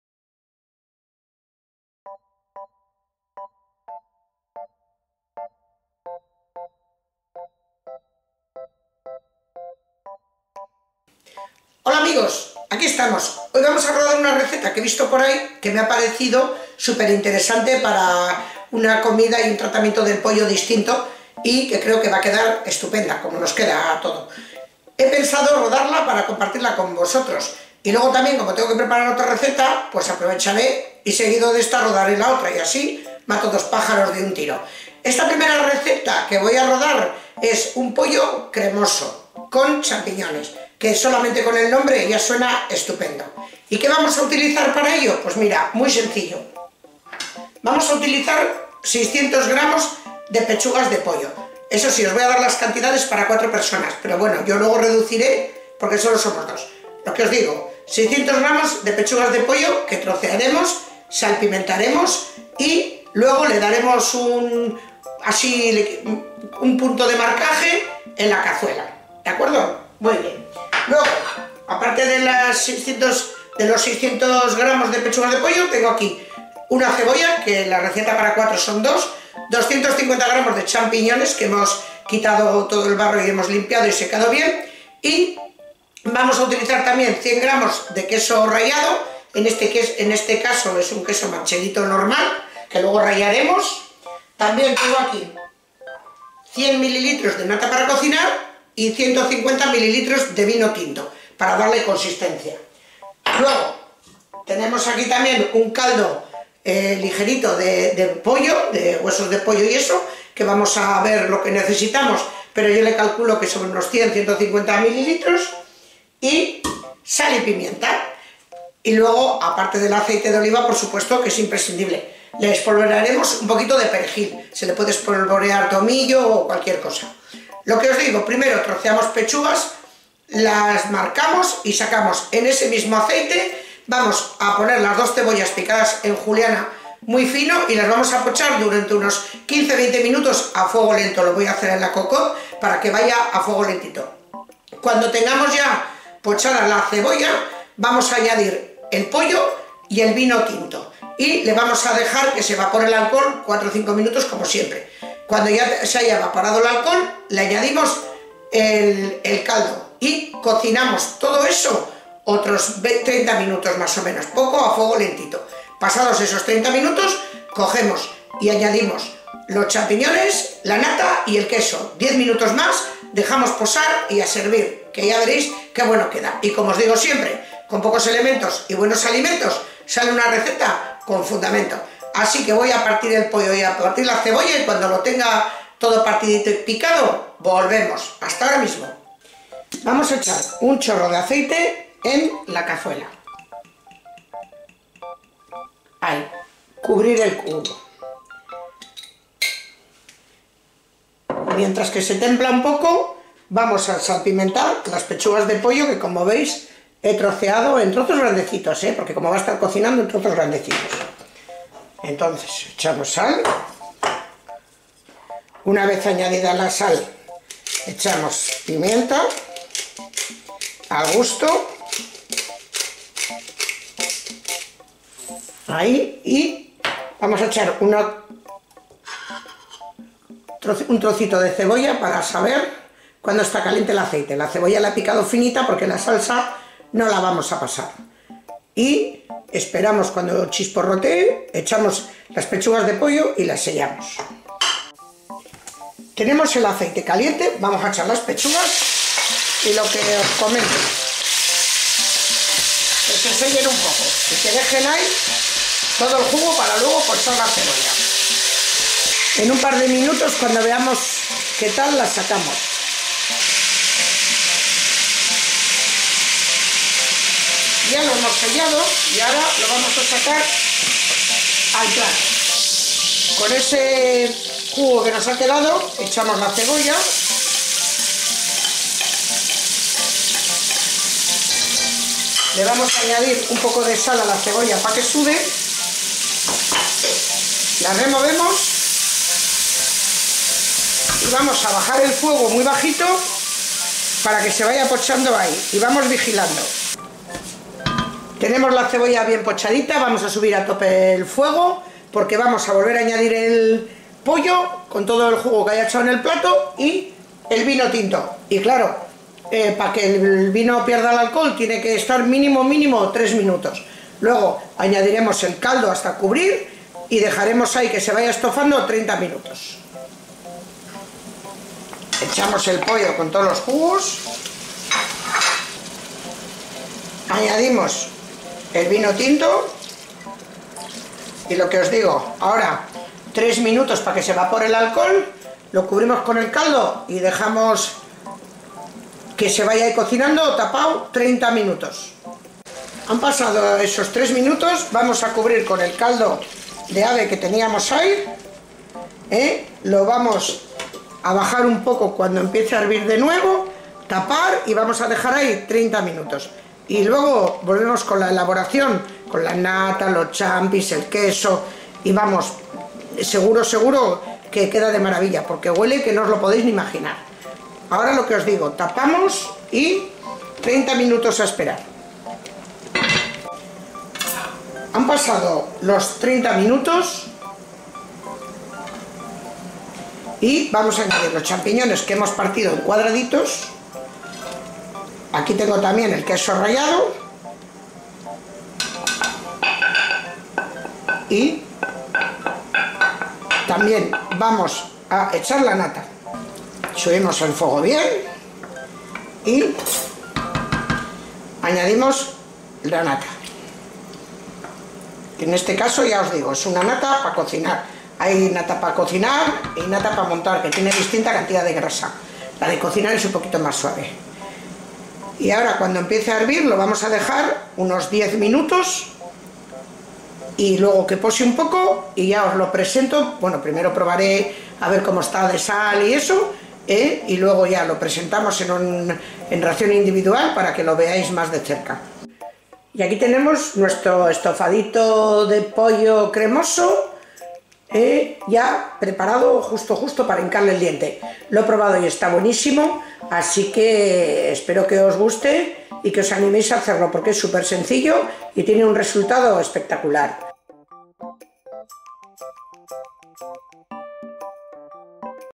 Pollo Cremoso con Champiñones y Queso ‐ Hecho con Clipchamp
Un receta que quedo muy , muy rica. Facil y distinta . En los ingredientes del video que pongo al final, falta el queso.
INGREDIENTES:
600 GR DE PECHUGAS DE POLLO,
2 CEBOLLAS
250 GR DE CHAMPIÑONES
100 GR DE QUESO TIPO MANCHEGO TIERNO.
100 ML DE CALDO DE AVE
100 ML DE NATA PARA COCINAR
150 ML DE VINO TINTO
SAL, PIMIENTA, ACEITE OLIVA VIRGEN